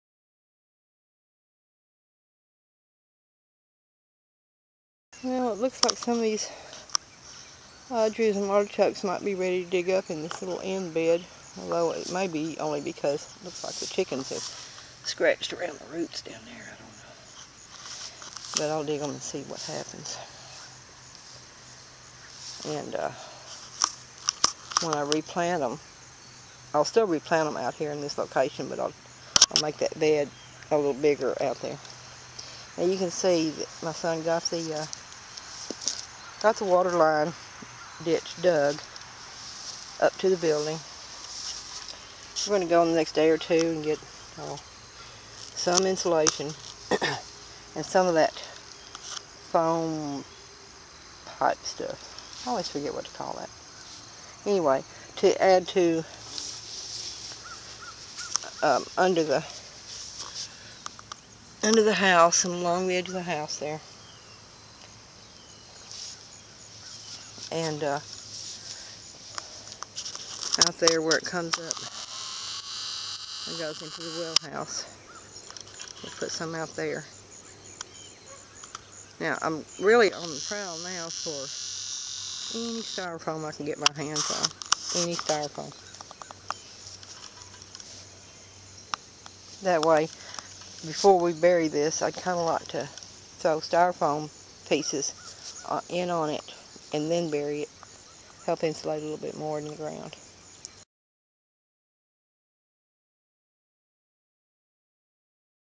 Water line update
It's getting closer. I ordered a remote switch today so that I can turn the pump off and on from the cabin in the event there's real bad weather.